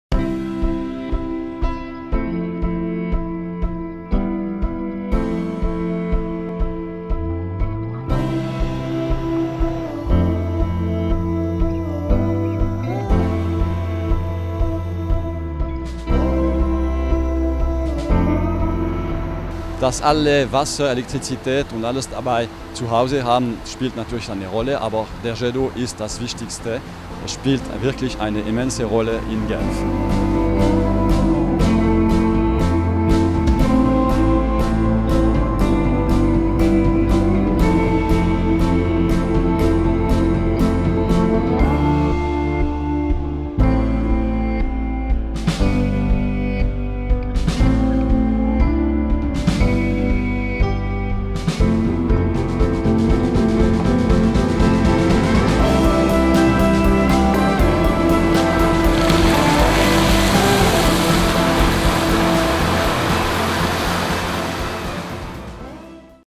Fonte de Genebra completa 125 anos
O “Jet d'Eau” é uma das atrações mais famosas de Genebra, no entanto, não foi originalmente construído para os turistas. A sua história está ligada à indústria relojoeira suíça. (SRF/RTS/swissinfo.ch)